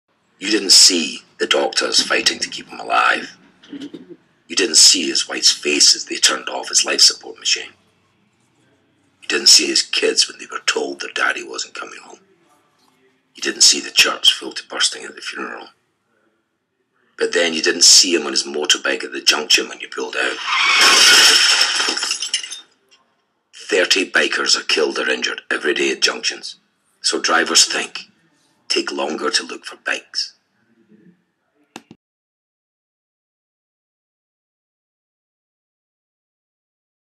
Think Bike
30 bikers are killed or injured PER DAY at junctions on GB roads.

Take longer to look for bikers